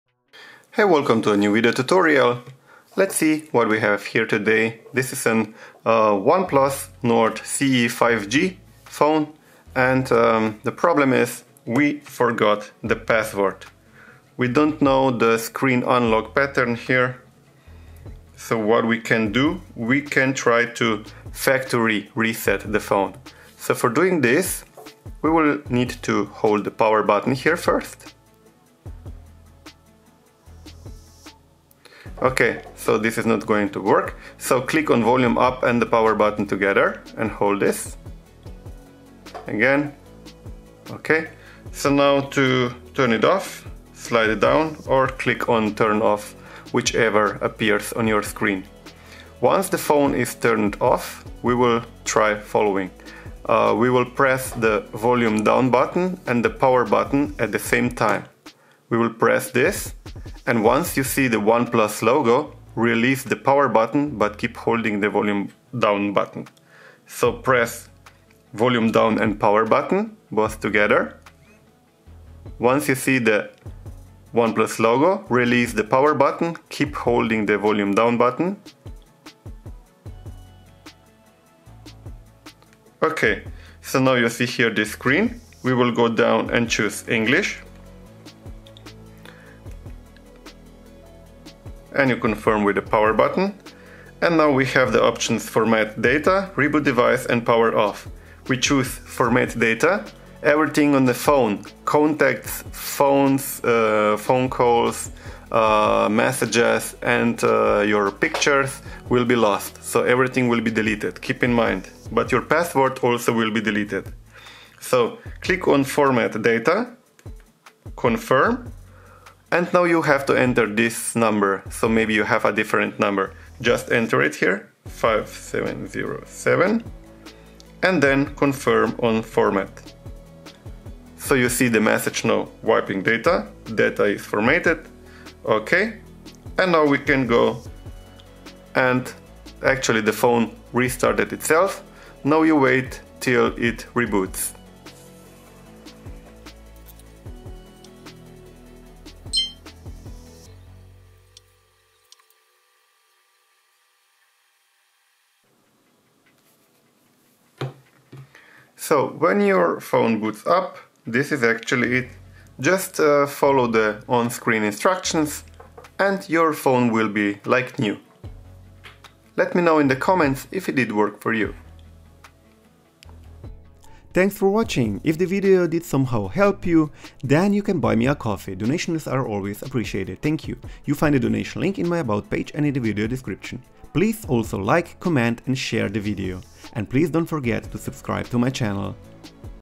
Oneplus Nord CE 5G - Remove Screen Lock / Factory Reset / Unlock
If you forgot your PIN, Password or another SCREEN Lock on your OnePlus Nord Smartphone , then this video is for you.

Note: All data will be lost/erased and if you had a google account on it, you will have to enter your email and password after reseting it.